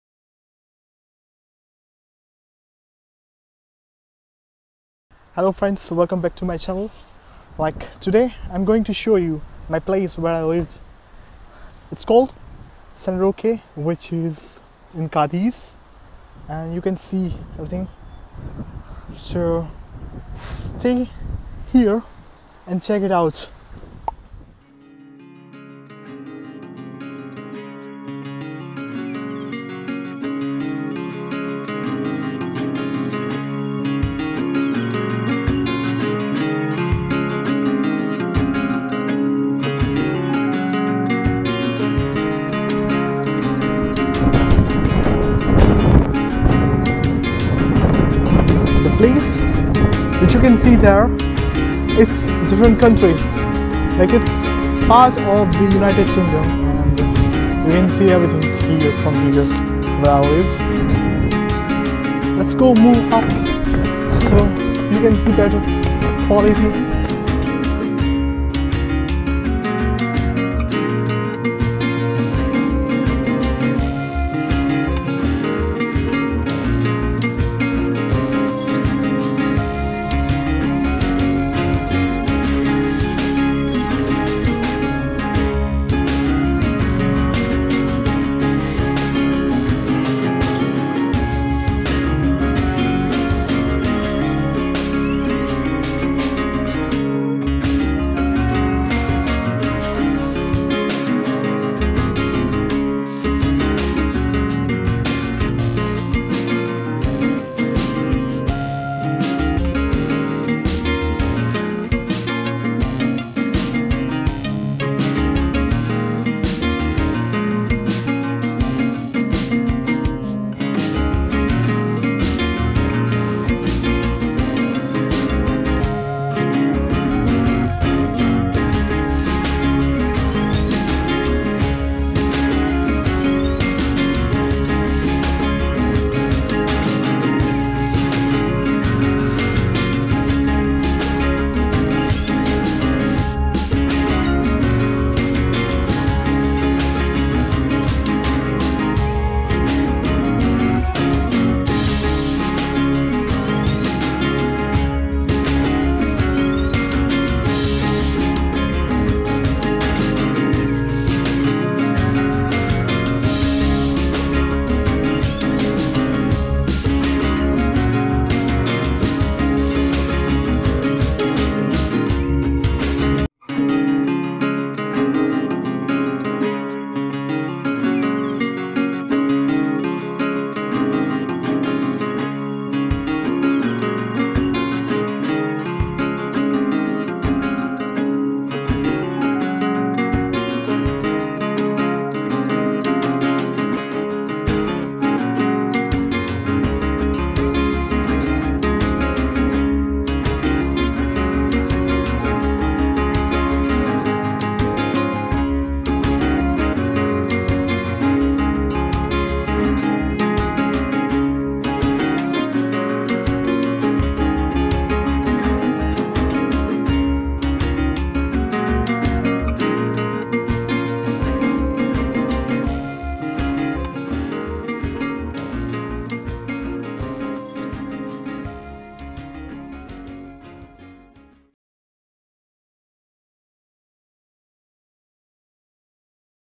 San roque - Cadiz| Beautiful place in spain | spain landscape.
In this video i shooted in san roque small town situated in Cadiz, spain. san roque is small and very beautiful town. it has really good landscape. i must recommending to visit this place once. it has everything what you required you want like.. Good restaurant, cafe shops, fast food, park, night life, good landscape, market and many more.
San roque is situated very near to la linea and close to gibraltar like 10 min by car. It has san roque golf resorts here. so if you like golf this place is for you. this place is also very near to the marbella 40 min by car and you can find accommodation here very affordable price.

I hope you like this video. and thank you for loving and support for our channel. we will always entertaining you.